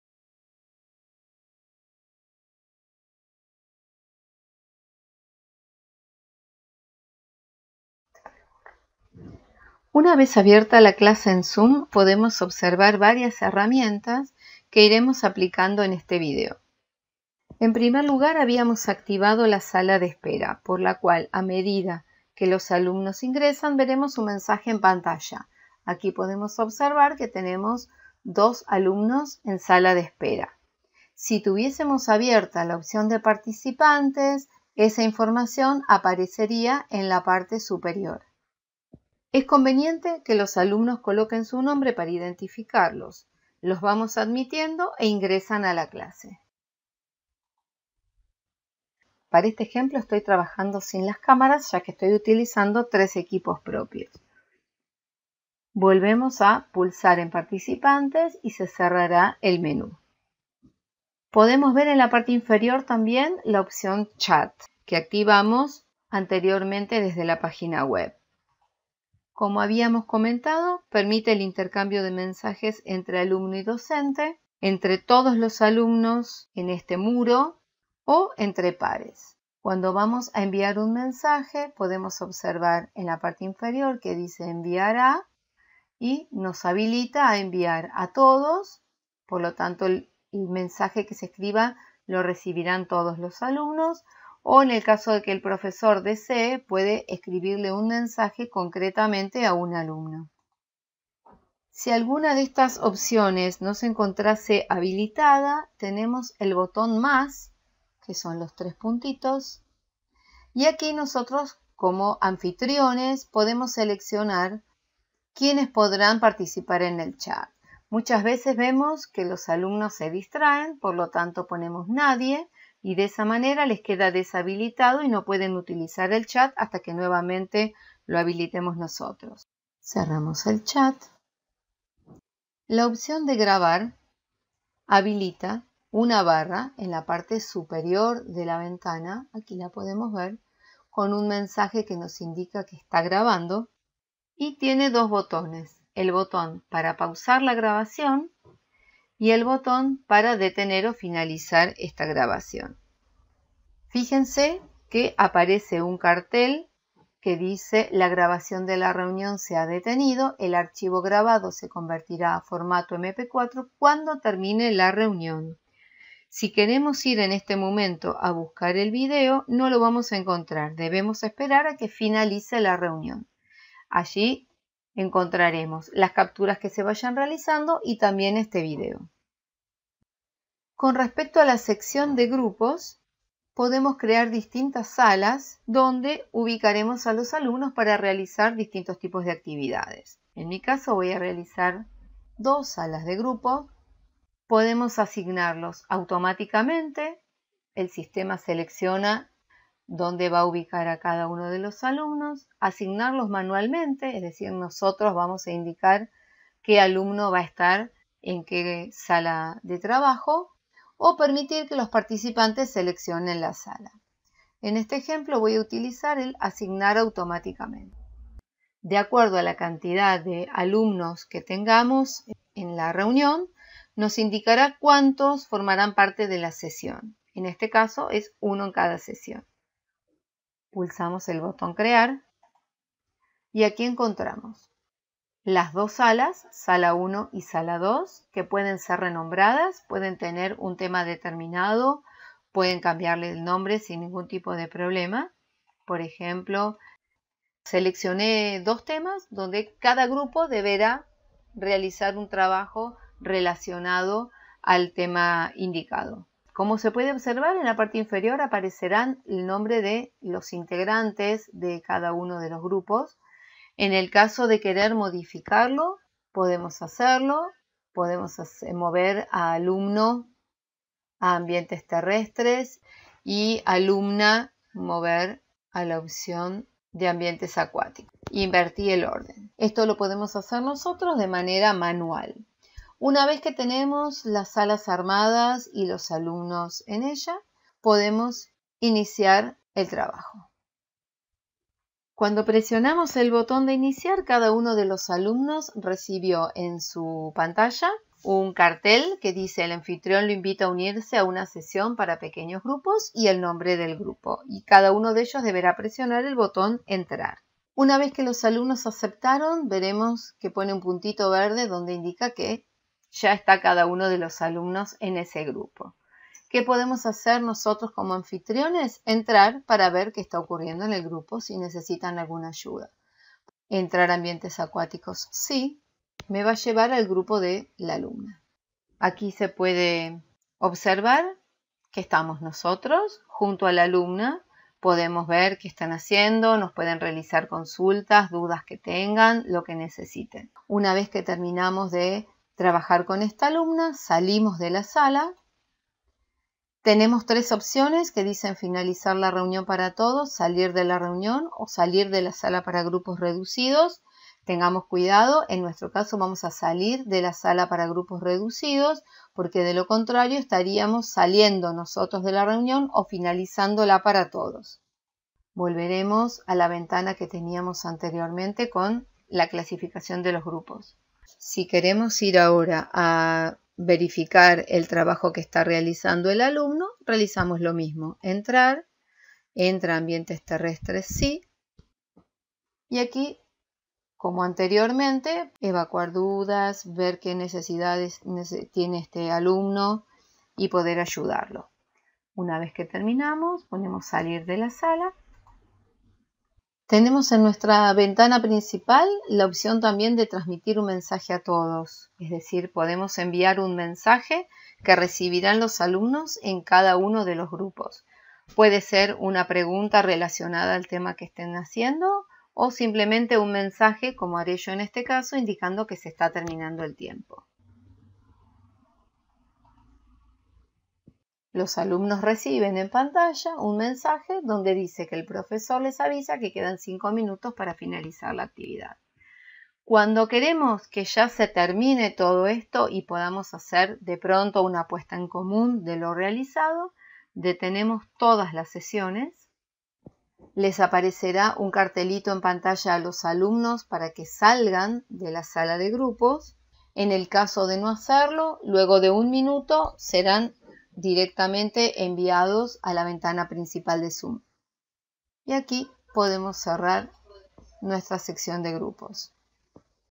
2. Zoom: Herramientas básicas
Este videotutorial permite visualizar las herramientas básicas de Zoom.
Aportes de la comunidad.